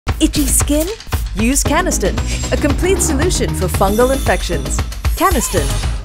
Canesten - A Complete Solution for Fungal Infections
Don't let fungal itches ruin your active lifestyle! Apply Canesten cream - your complete solution to stop fungi growth, kill fungi and relieve symptoms.